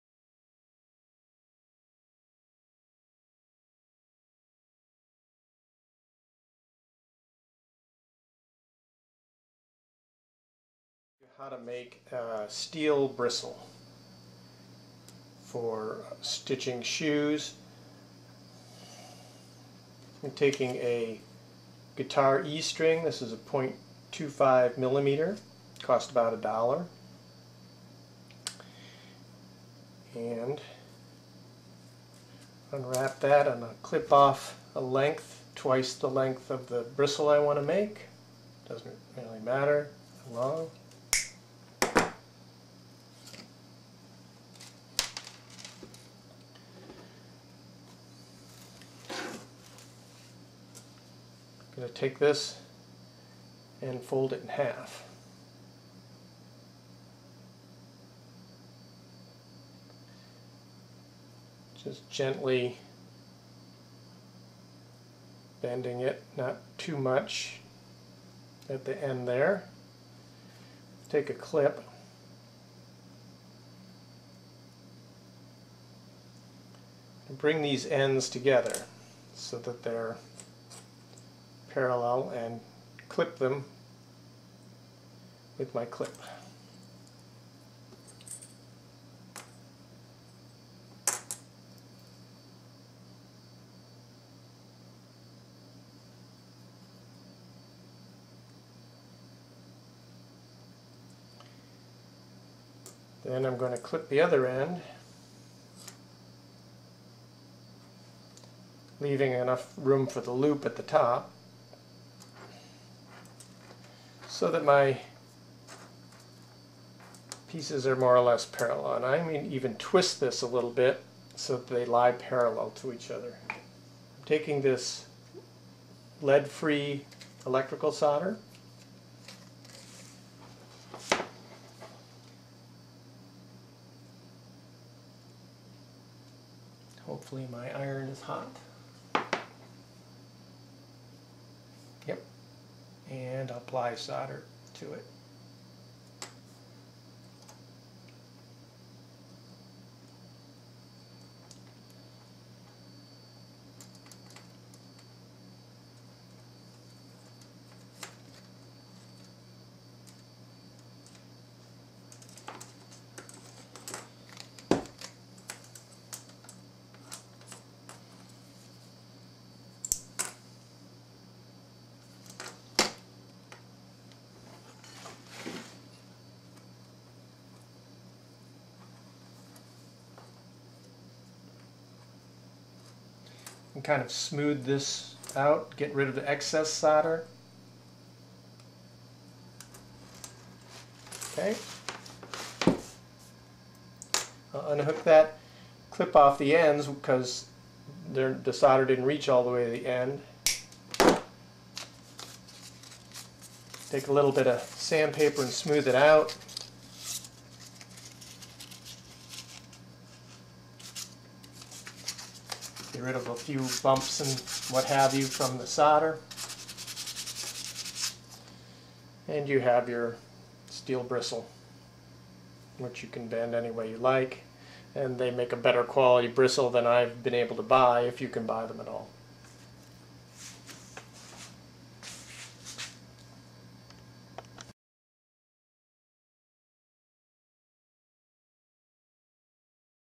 Making Steel Bristles.wmv
How to make excellent steel bristles from guitar E strings.